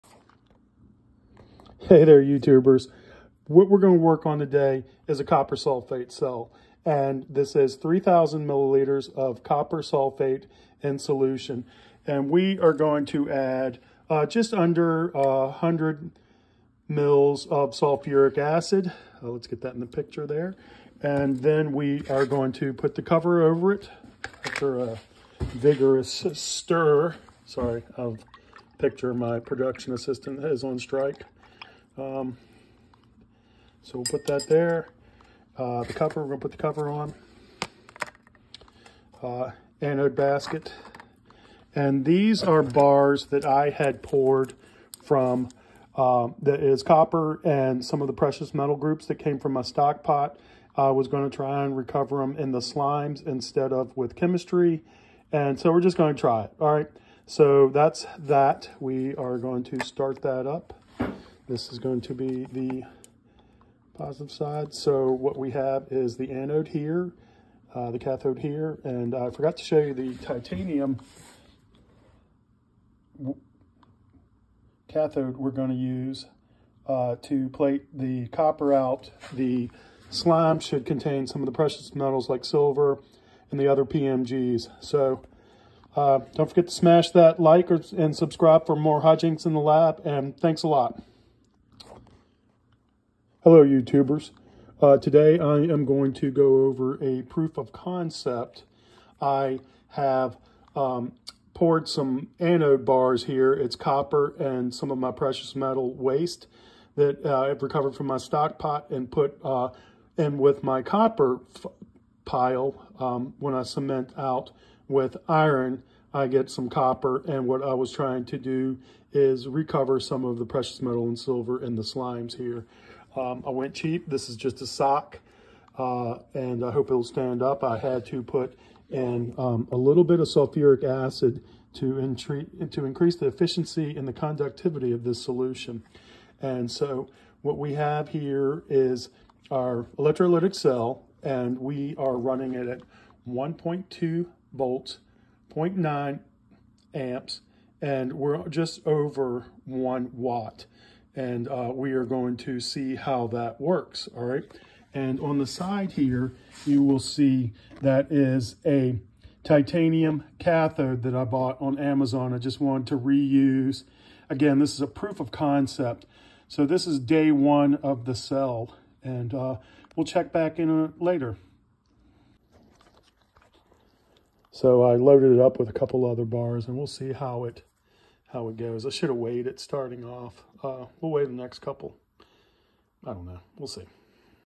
Copper Cell start up
This is copper sulphate cell . Its electrolyte is a solution of water and Copper Sulphate with an addition of sulfuric acid. I will be using this to recover precious metals from my Stock pot .

If You do this, you do so at your own risk.  I assume no responsibility for your actions